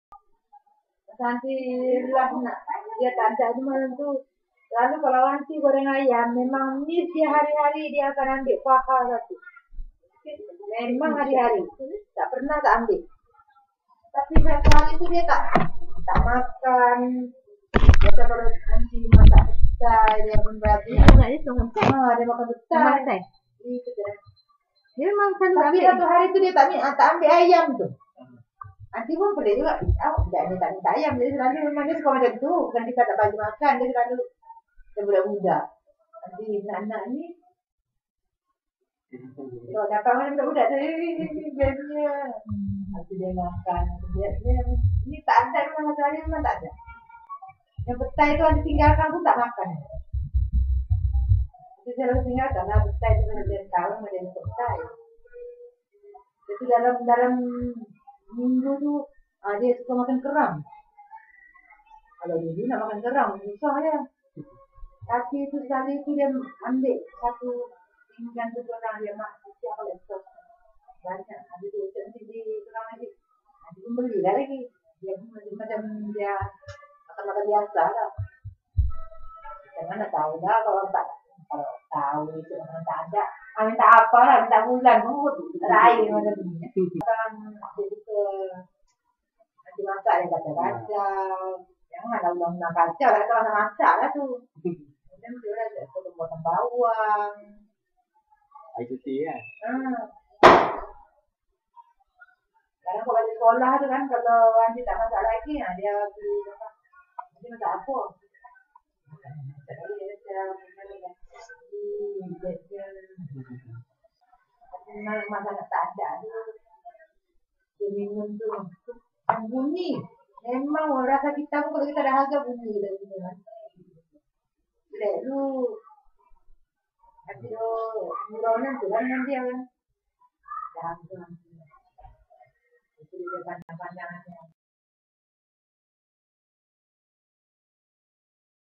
Love Yasmin : Behind The Scene
Sembang santai bersama Aunty Saz dan Aunty Dina semasa kunjungan pertama saya ke rumah Yasmin di Langkawi..